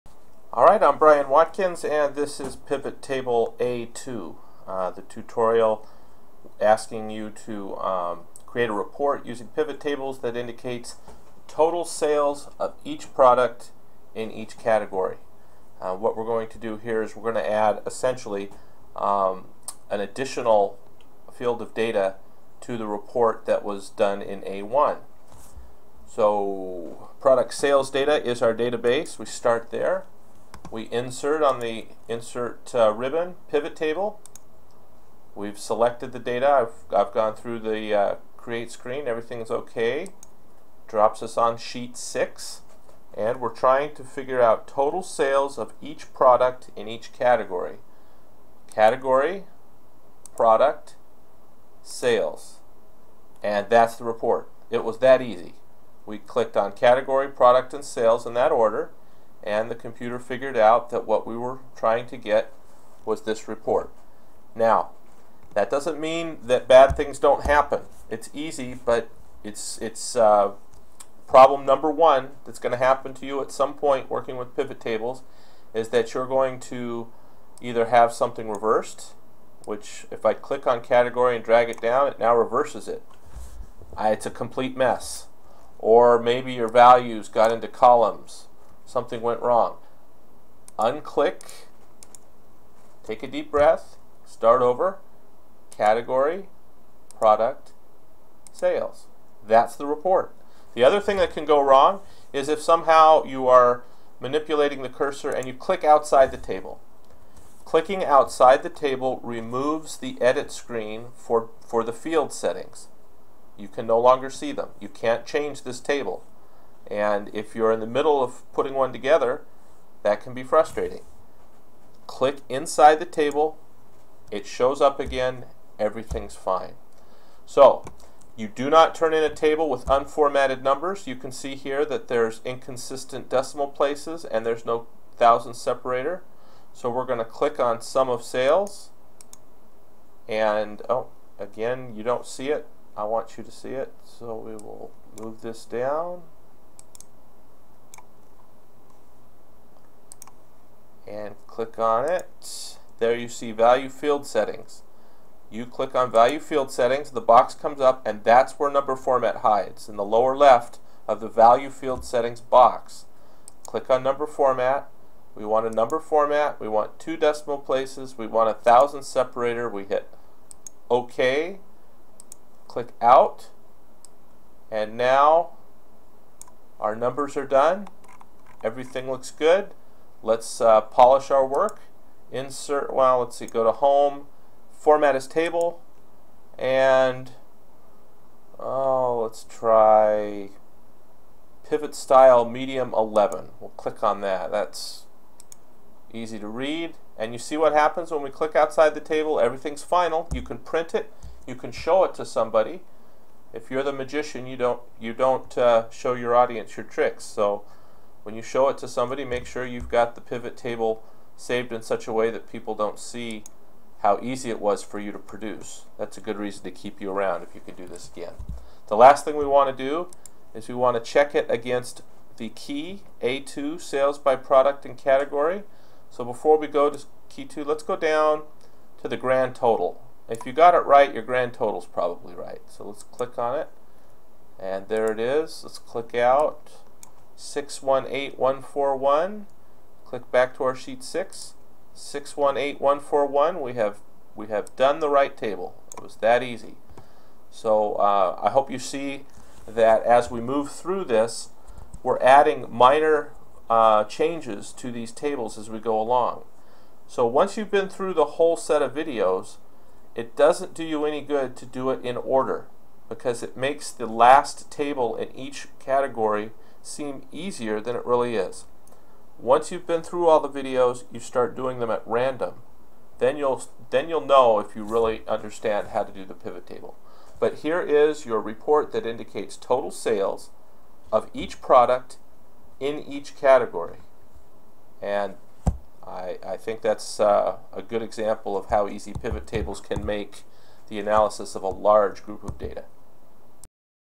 Pivot Table Example A2
This tutorial demonstrates how to produce a Pivot Table showing total sales organized by two fields, category and product.  It also demonstrates how to format a finished product.